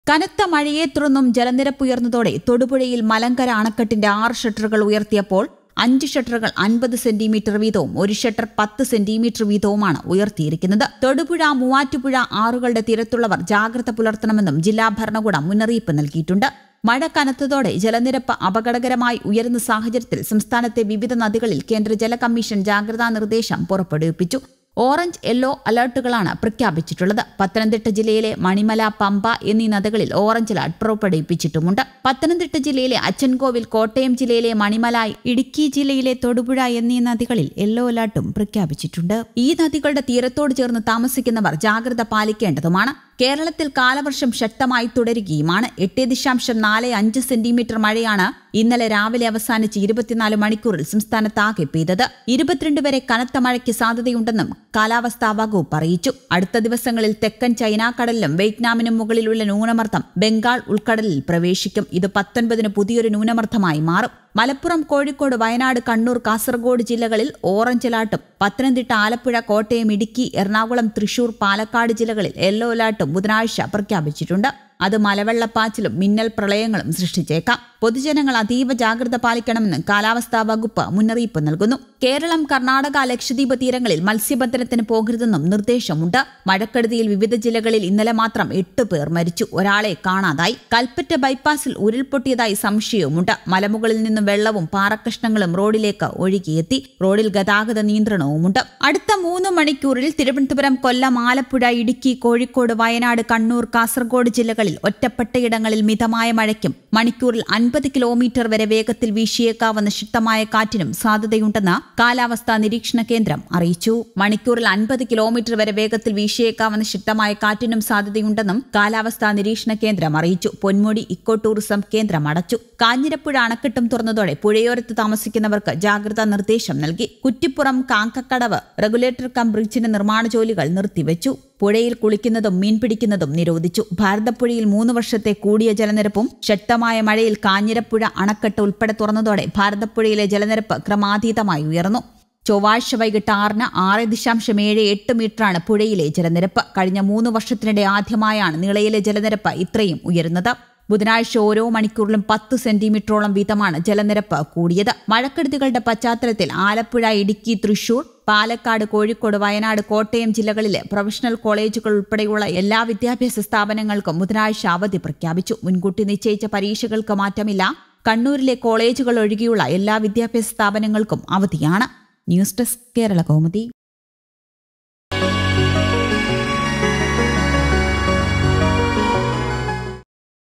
അതിതീവ്രമഴ മുന്നറിയിപ്പ്, നദികളില്‍ ജലനിരപ്പ് ഉയരുന്നു, ഉരുള്‍പൊട്ടല്‍ | Kerala Rains
കനനത്ത മഴയെത്തുടര്‍ന്നു ജലനിരപ്പുയര്‍ന്നതോടെ തൊടുപുഴയില്‍ മലങ്കര അണക്കെട്ടിന്റെ ആറു ഷട്ടറുകളും ഉയര്‍ത്തിയപ്പോള്‍. 5 ഷട്ടറുകള്‍ 50 സെന്റിമീറ്റര്‍ വീതവും ഒരു ഷട്ടര്‍ 10 സെന്റിമീറ്ററുമാണ് ഉയര്‍ത്തിയിരിക്കുന്നത്. തൊടുപുഴ, മൂവാറ്റുപുഴ ആറുകളുടെ തീരത്തുള്ളവര്‍ ജാഗ്രത പുലര്‍ത്തണമെന്നു ജില്ലാഭരണകൂടം മുന്നറിയിപ്പു നല്‍കിയിട്ടുണ്ട്.